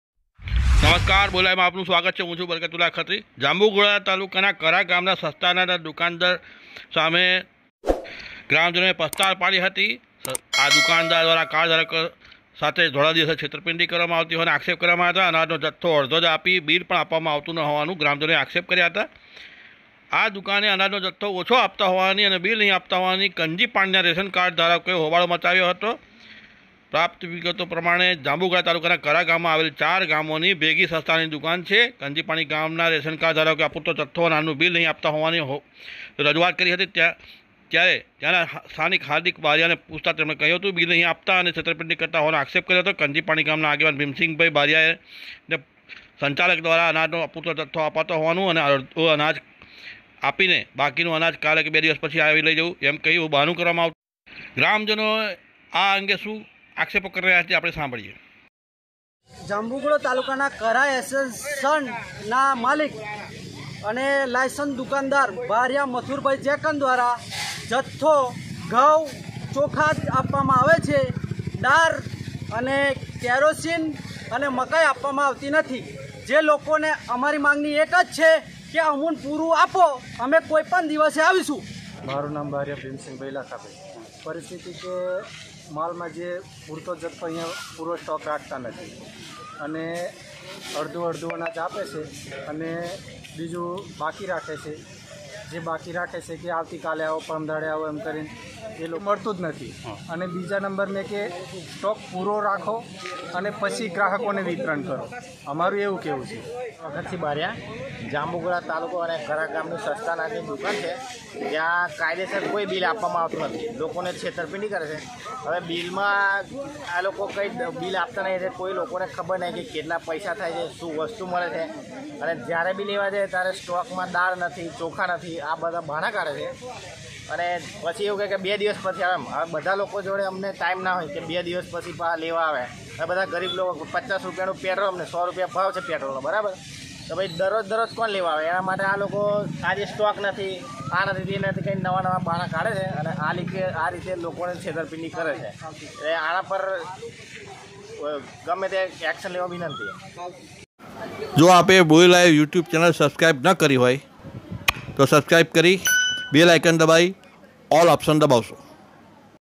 જાંબુઘોડા- કરા ગામની રેશનીંગ દુકાને ગ્રામજનોનો હોબાળો,દુકાનદાર સામે શુ કર્યા આક્ષેપો?જુઓ બોડેલી લાઇવ
જાંબુઘોડાના કરા ગામની રેશનીંગ દુકાને ગ્રામજનોનો હોબાળો,દુકાનદાર સામે શુ કર્યા આક્ષેપો? જુઓ બોડેલી લાઇવ